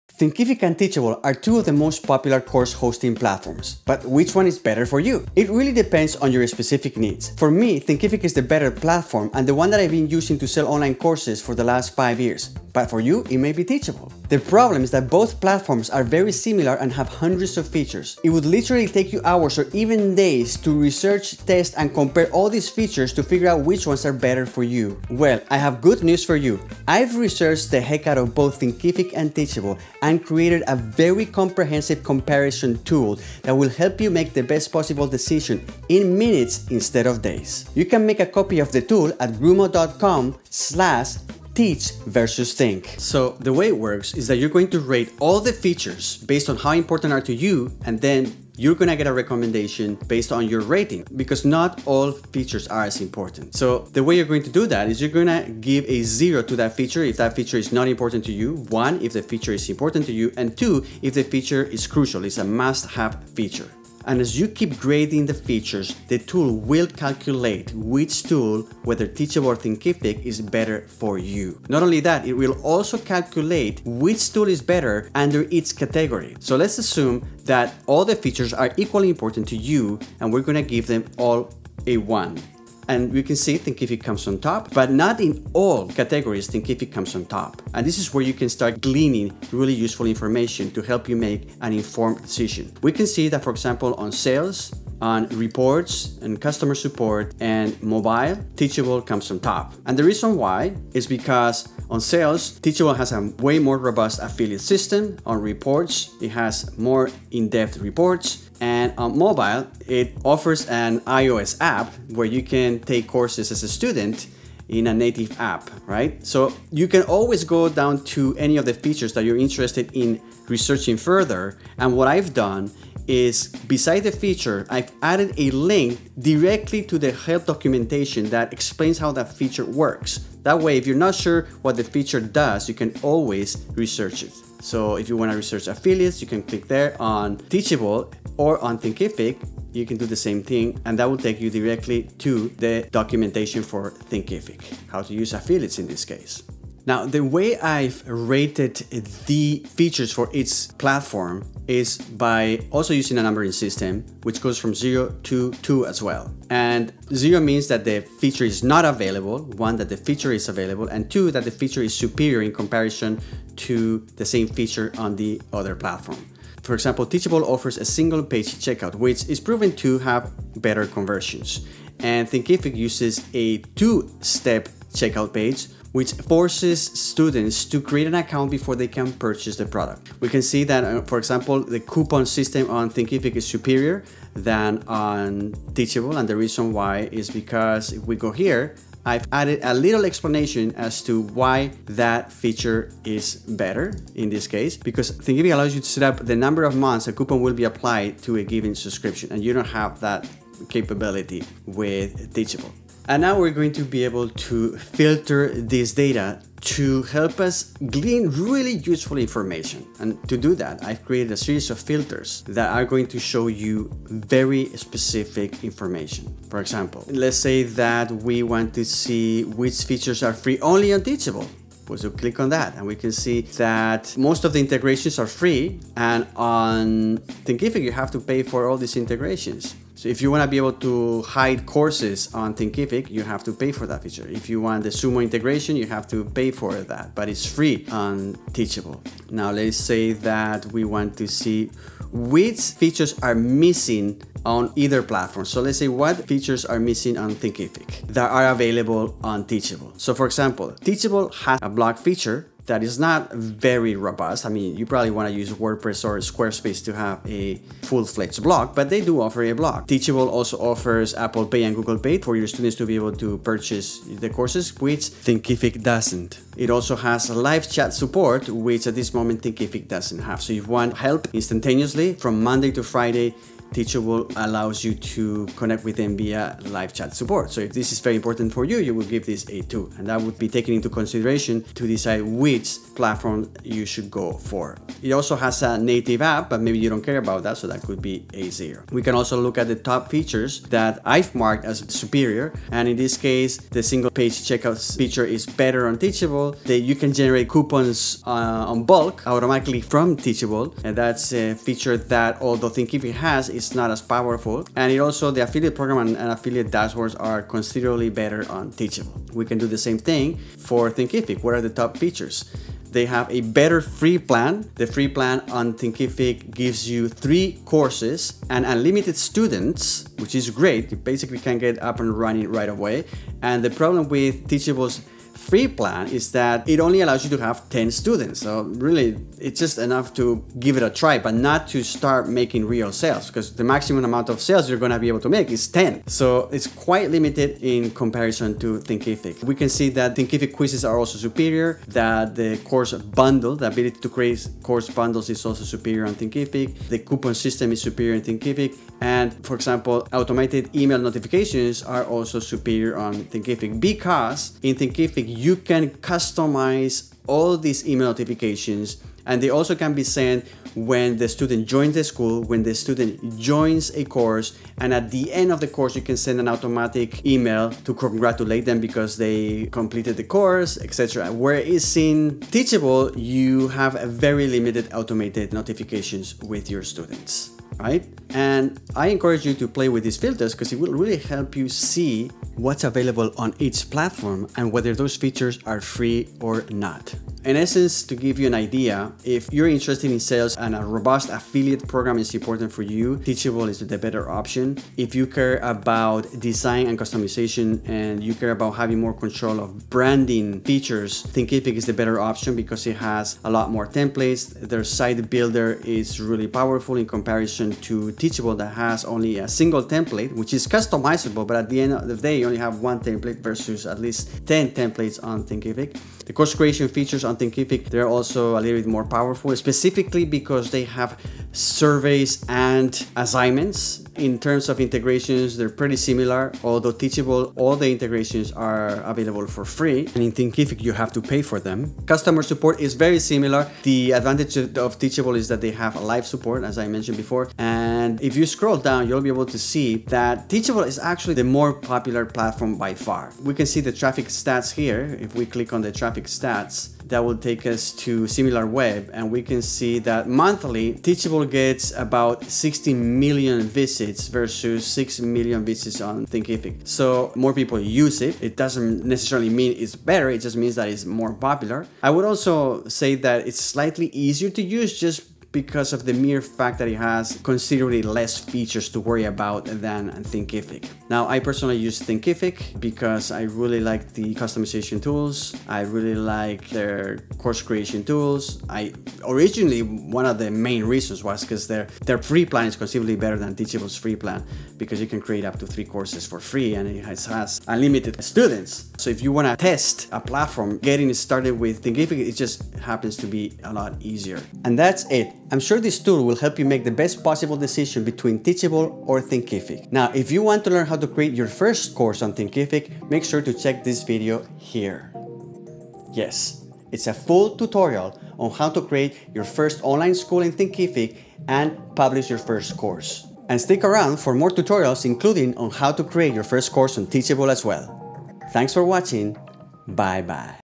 THINKIFIC vs TEACHABLE - Which one is BETTER?
Thinkific vs Teachable - The Ultimate Comparison - Download the comparison tool

00:00 - Why is so hard to compare them?
00:26 - Download the comparison tool
00:42 - Learn to use the comparison tool
01:16 - Rating all the features
01:32 - Thinkific vs Teachable results comparison
01:55 - Links to features documentation
02:26 - My rating system for all the features
02:46 - An example where Teachable is superior
02:59 - An example where Thinkific is superior
03:22 - Using built-in filters to compare features
03:59 - Features missing on Thinkific
04:48 - Top features on Teachable
05:09 - Top features on Thinkific
06:27 - Summary of strengths and weaknesses for each platform
07:18 - Which platform is the most popular?
07:41 - Which platform is easier to use?
07:51 - Which platform do I prefer and Why?
08:14 - Learn how to create your first course on Thinkific

UPDATE: Teachable doesn't offer a free plan anymore :(

Miguel @ Grumo.com